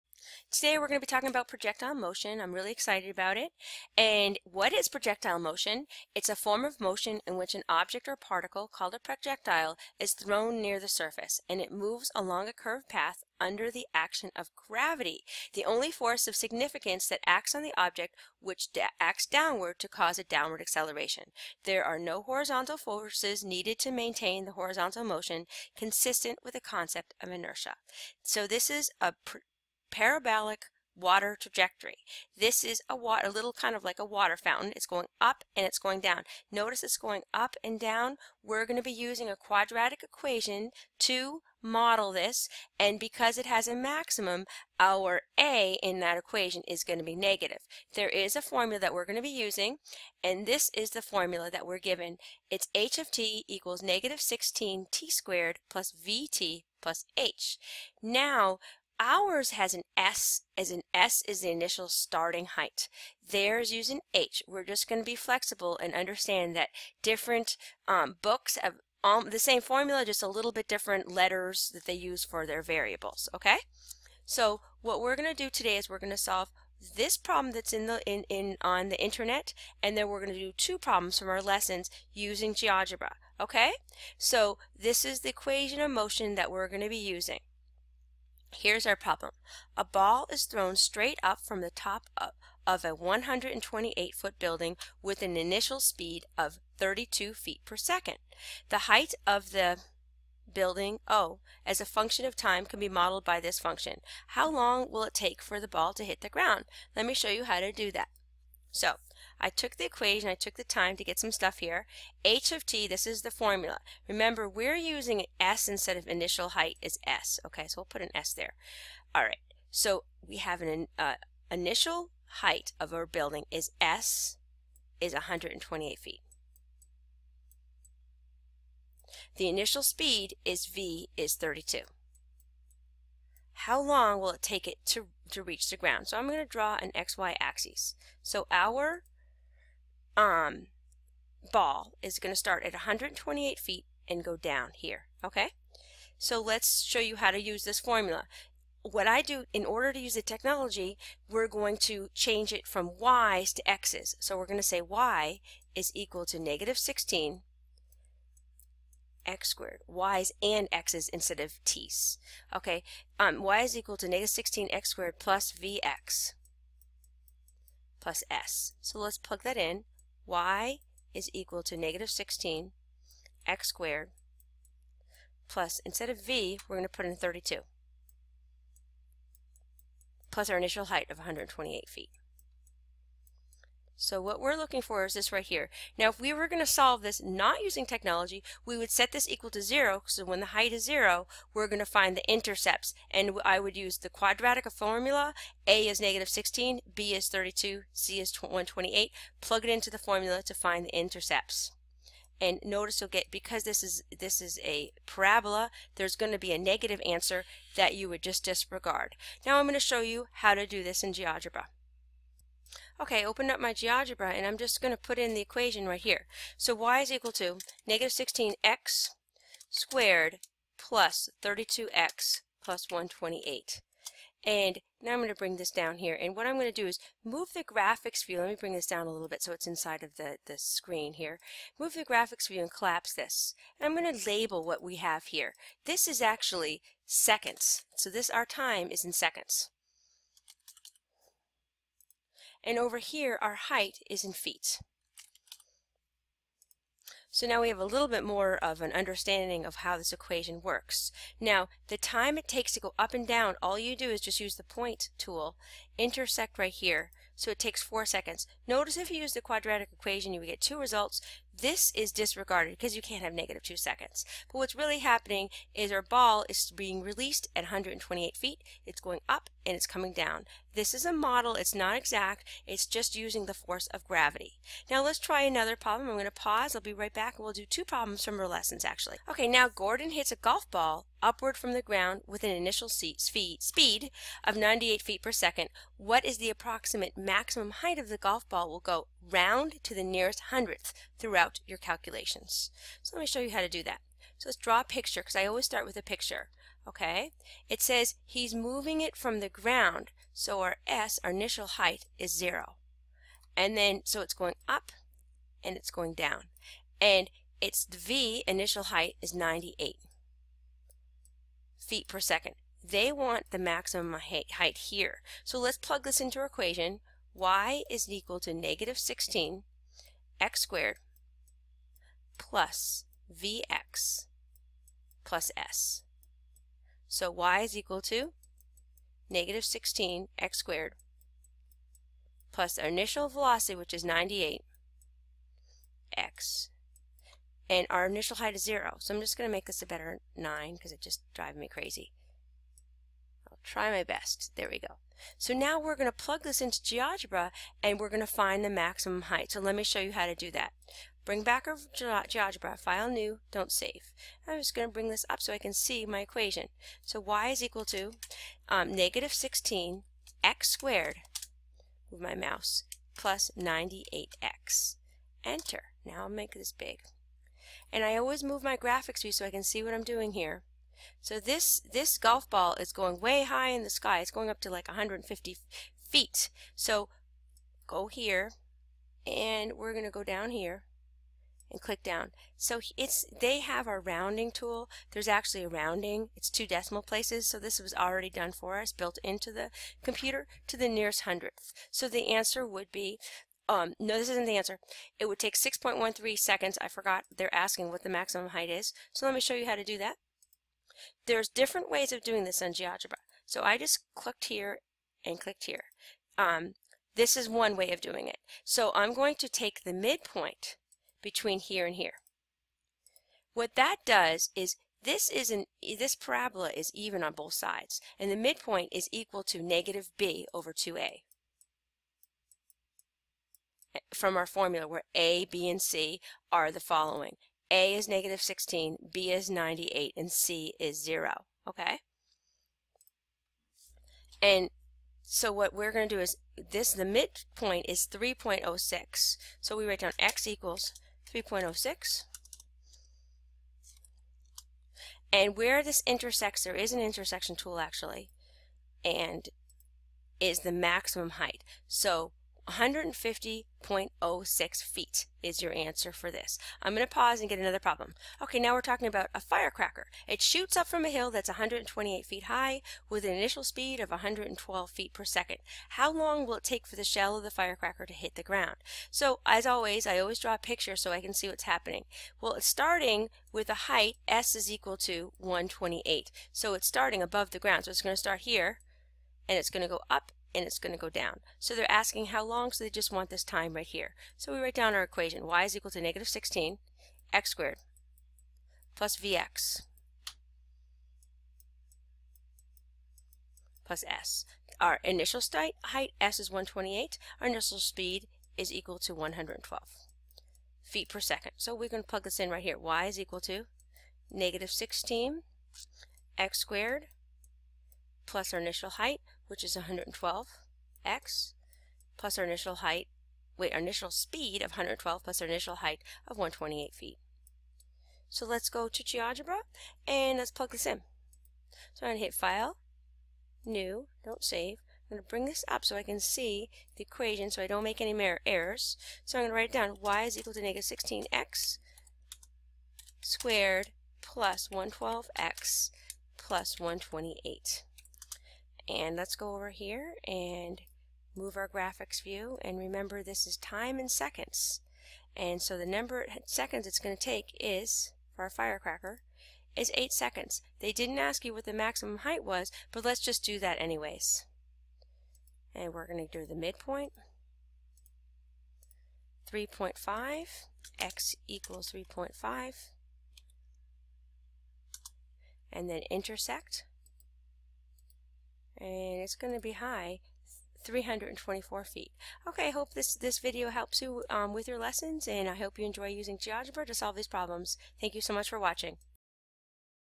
Projectile motion using Geogebra
How to use geogebra to help you solve Projectile Problems using the formula: h(t) = -16t^2 + vt + s